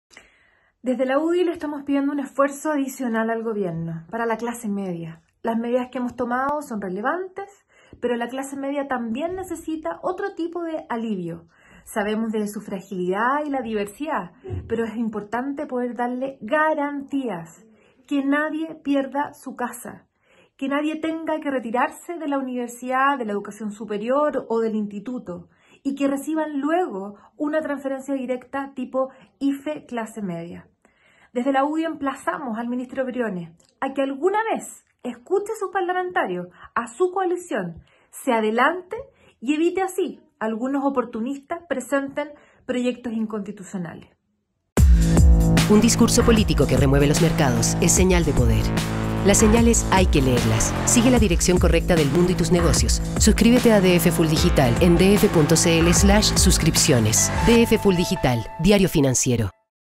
UDI emplaza al ministro Briones por IFE: "Pedimos garantías para la clase media"
Jefa de Bancada UDI, María José Hoffmann, por propuesta de Ingreso Familiar de Emergencia para la clase media.